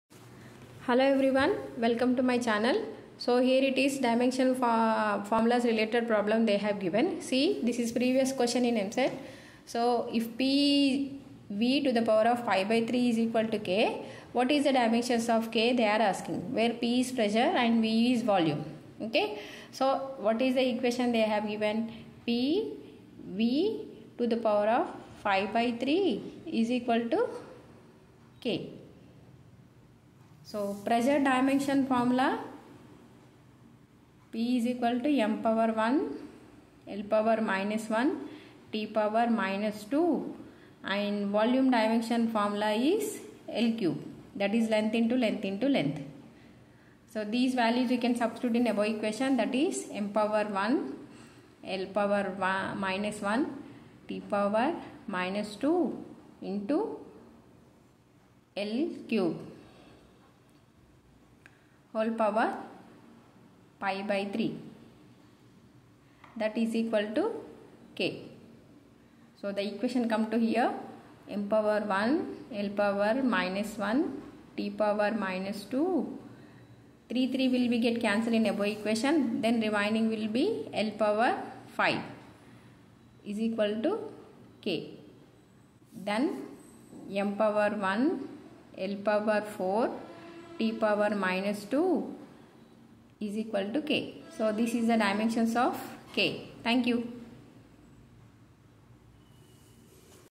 Previous eamcet questions on dimensional analysis part 1@physicsstudytime#dimensional analysis
@physicsstudytime9614 hello everyone in this video I have explained previous eamcet questions on dimensional analysis.thank u.